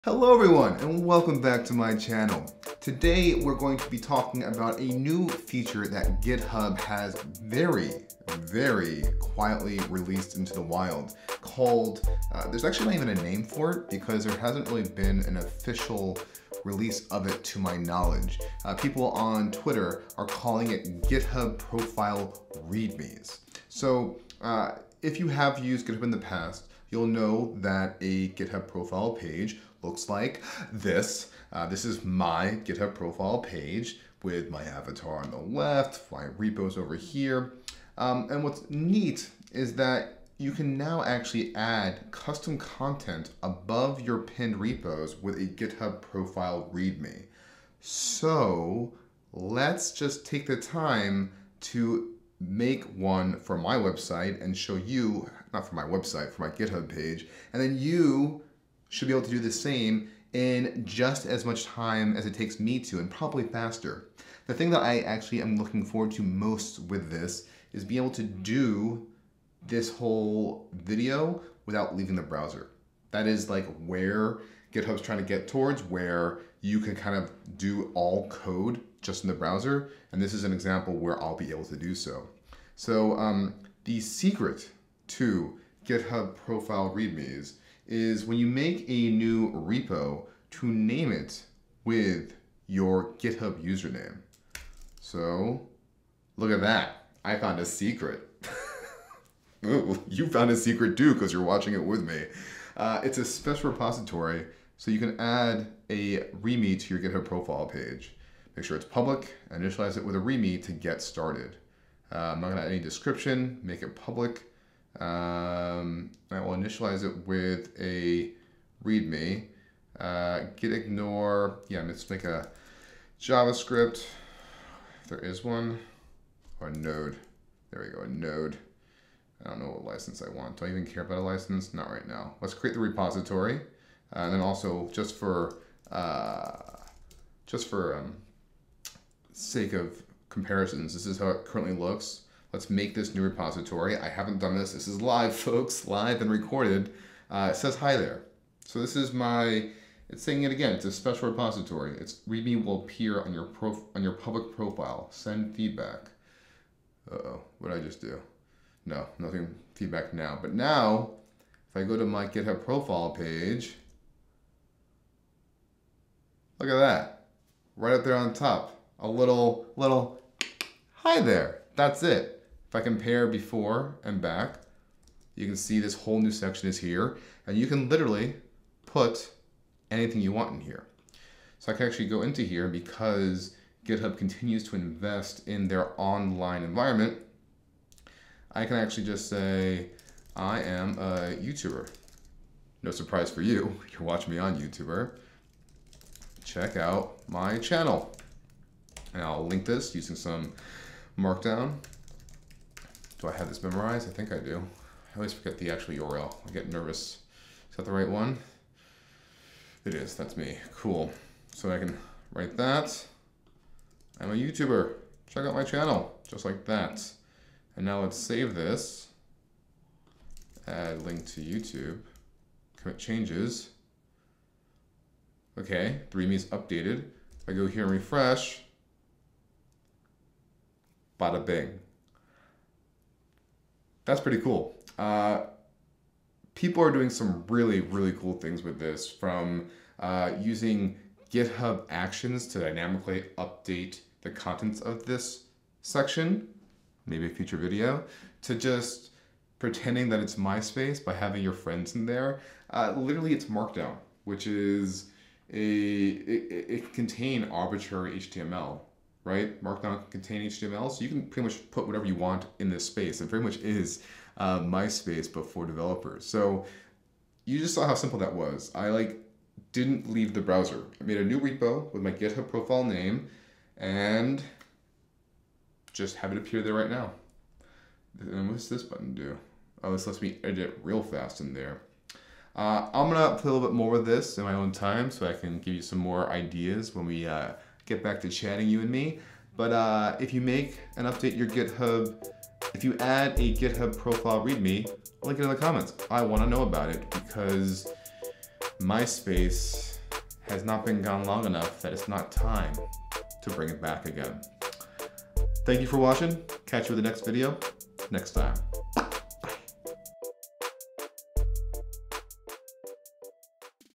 What are GitHub Profile READMEs??
GitHub has a secret...you can include a README on your profile page!

It's not widely known so I figured I'd let you in on the secret!

If you want this in word form check out this excellent blog post that gives you all the details